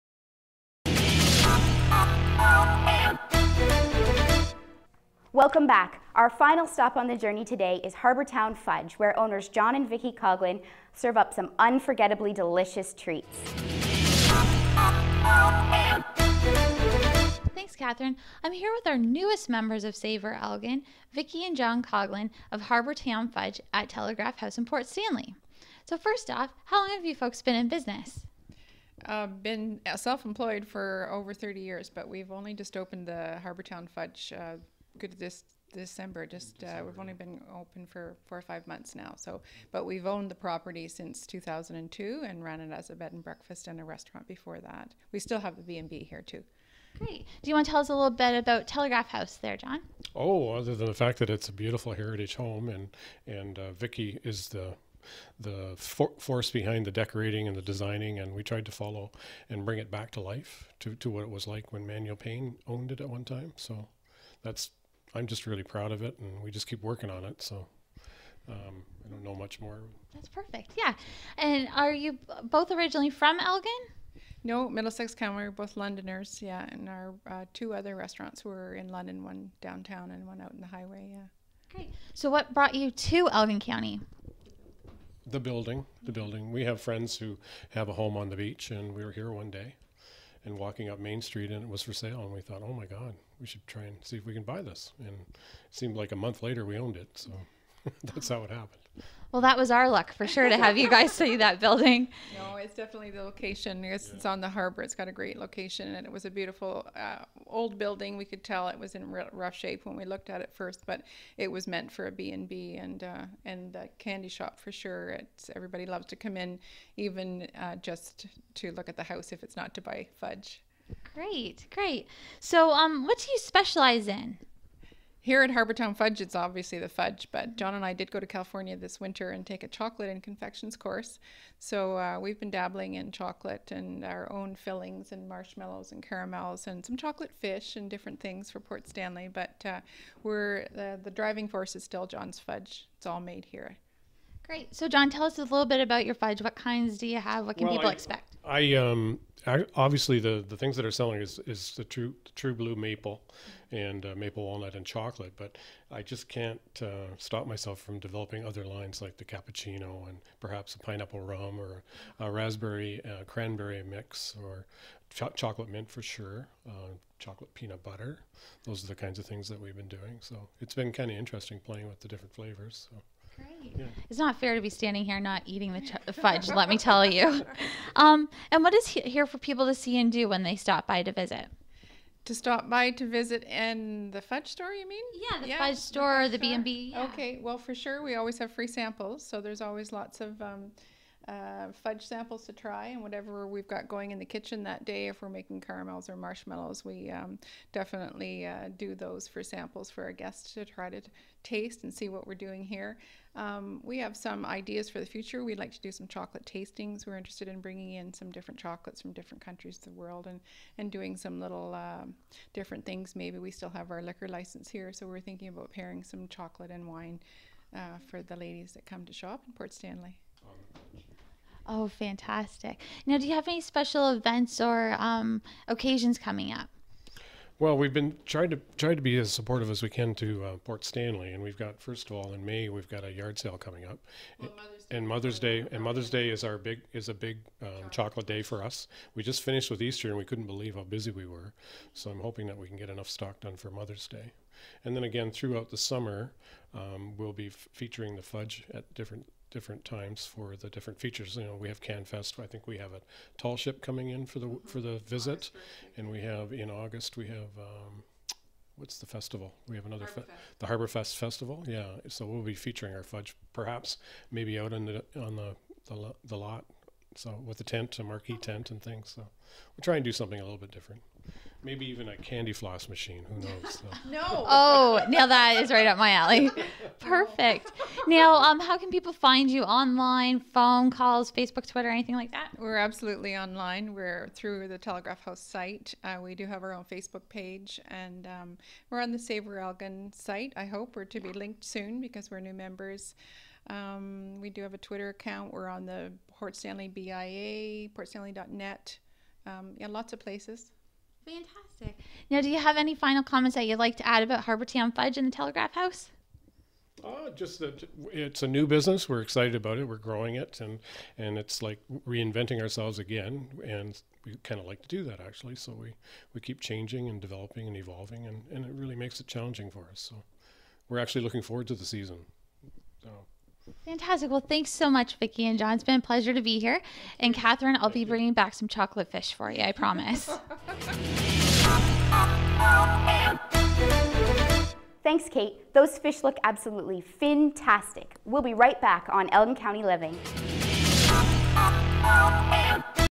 Elgin County Living Ep 1, Harbourtown Fudge
In the premiere episode of Elgin County Living we visit Savour Elgin members, Harbourtown Fudge